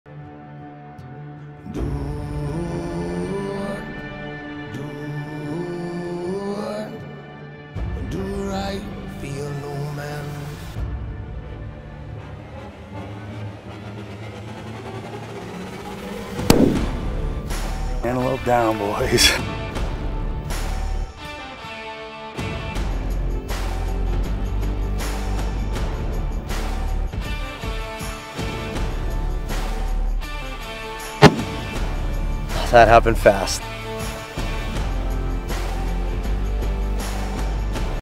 The Double Official Trailer- A Antelope Hunt Coming March 27th
Shane and Pops head to Wyoming looking to start their fall off filling their Wyoming Antelope tags